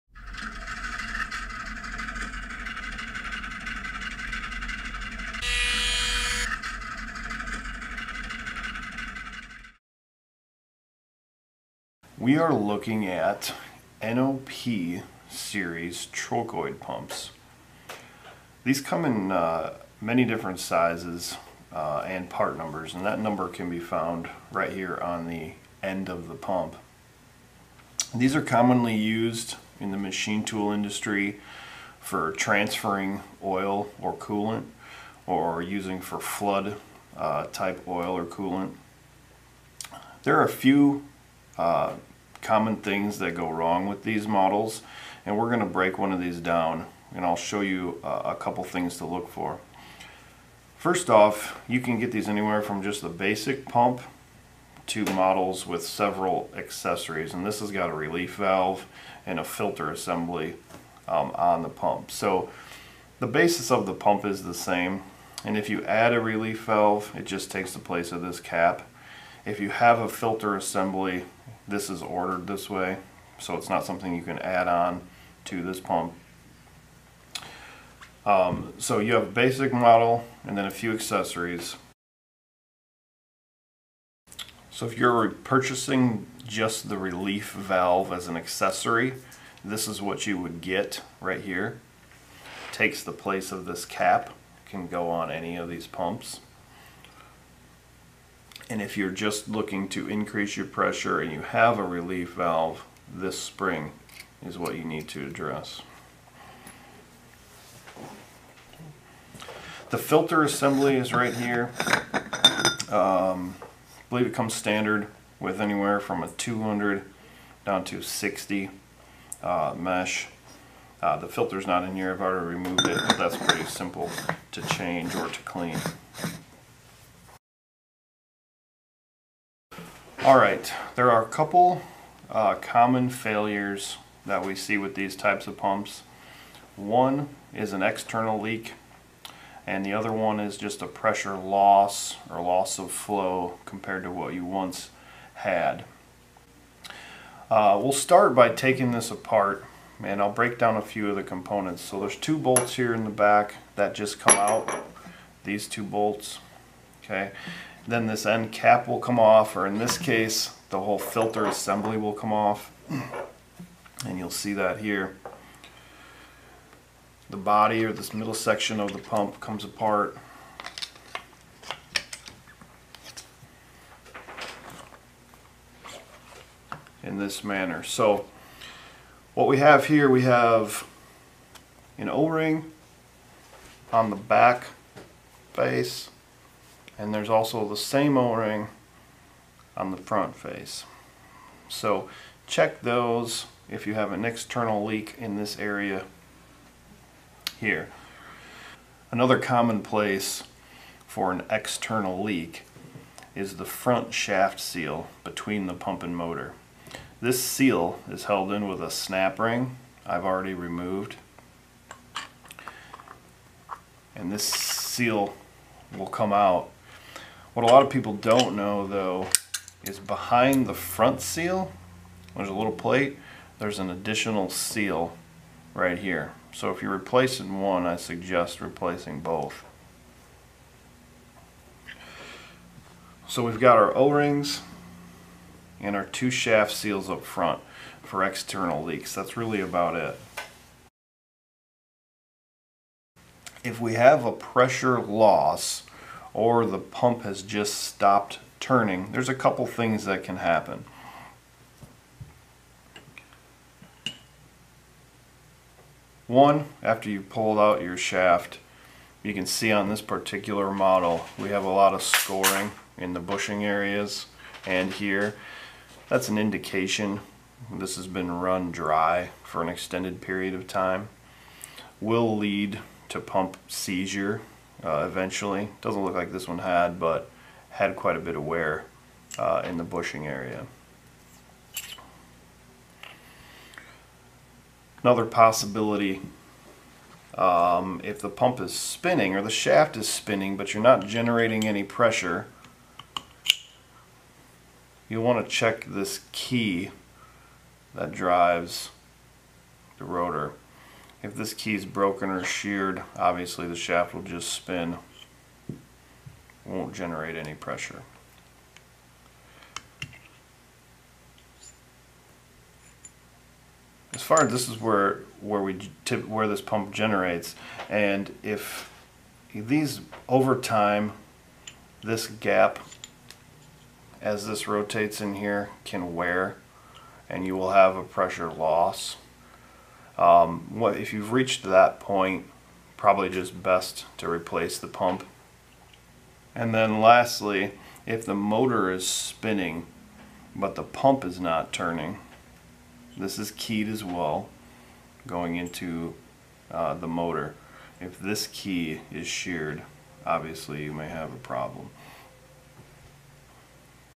Troubleshooting Trochoid Pumps
How to troubleshoot a pressure loss, external leak, accessory malfunction, or other failure on a trochoid pump, also known as a geroter pump. Model used is NOP.

0:00 Introduction
1:23 Accessory Failures
1:28 Pressure Adjustments
1:51 Filtration Issues
2:09 External Leaks
3:01 O-Ring Failure
3:20 Front Shaft Seal
4:02 Pressure Failures
4:18 Running Dry
4:46 Broken Pump Key
5:39 Broken Motor Key